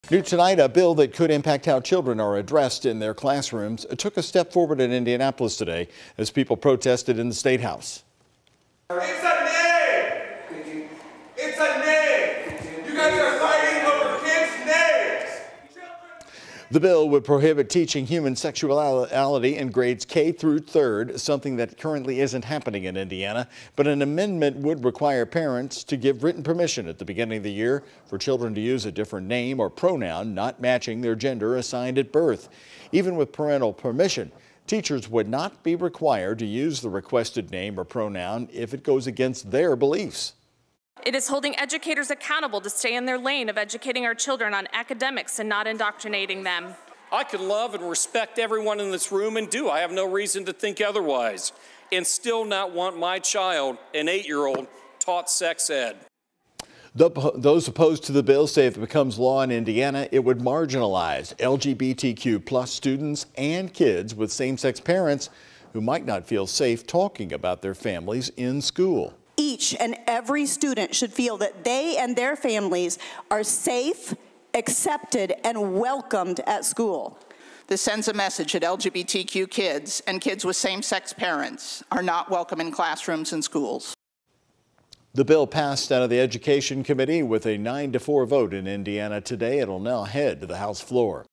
Indiana's 'pronoun' bill receives backlash from community
Under the proposed bill, schools could be required to tell parents if a student changes their gender identity or preferred name.